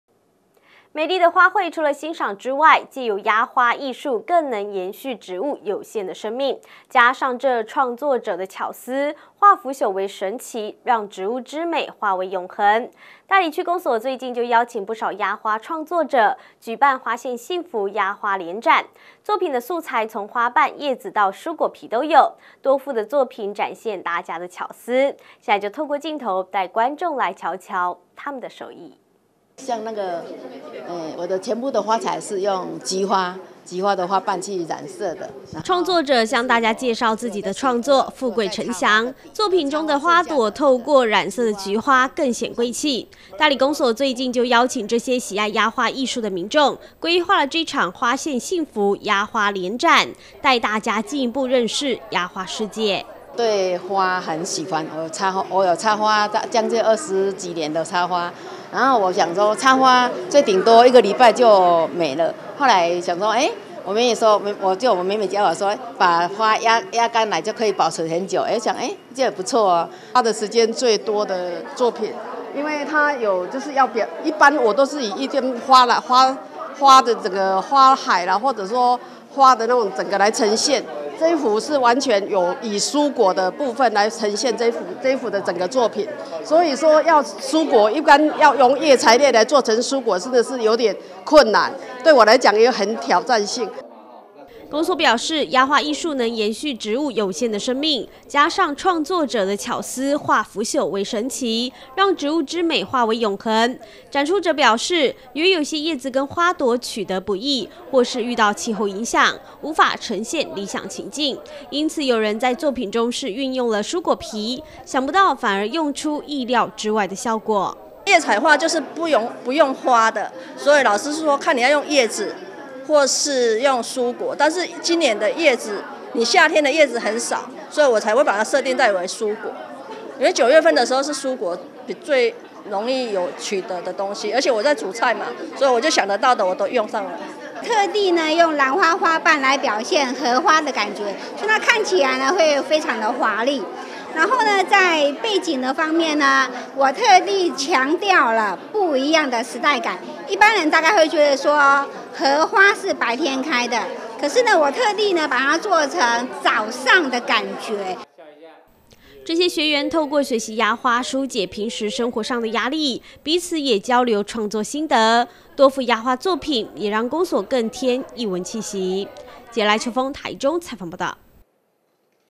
大台中新聞 大里公所押花展
押花藝術能延續植物有限的生命，加上創作者的巧思，化腐朽為神奇，讓植物之美化為永恆，大里區公所最近就邀請不少押花創作者，舉辦「花現幸福，押花聯展」，作品的素材從花瓣，葉子到蔬果皮都有，多幅作品展現大家的巧思，現在就透過鏡頭，帶觀眾瞧瞧她們的手藝。「以上新聞可從哈TV APP收視」